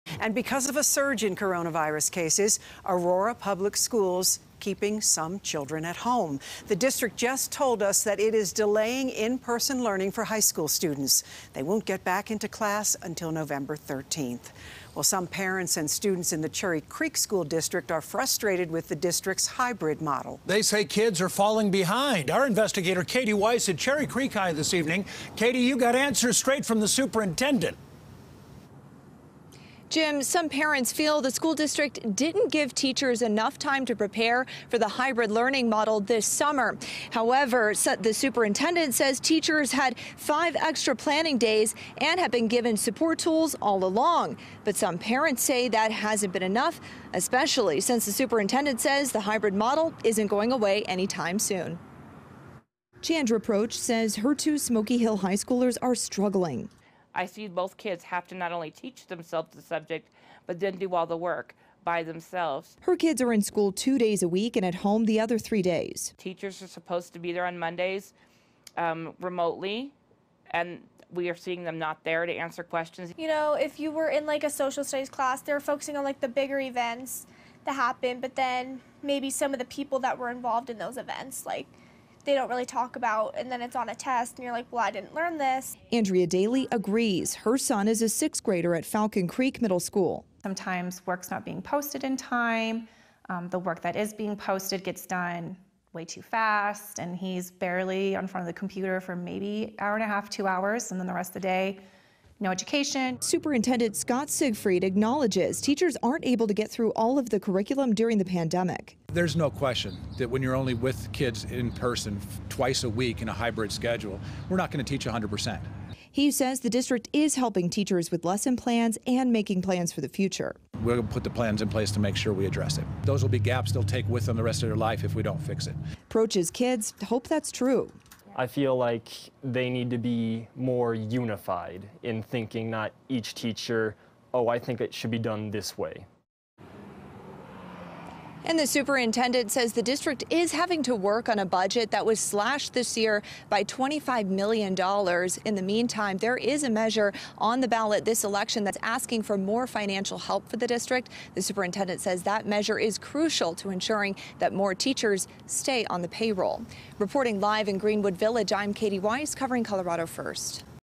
Some Cherry Creek School District Students Falling Behind During Hybrid Learning: 'It's Frustrating
Some parents and students in the Cherry Creek School District are frustrated with the district's hybrid model, saying kids are falling behind. While the superintendent says teachers have been given several support tools for hybrid lessons plans, some parents feel the school district should be doing more to help students and teachers.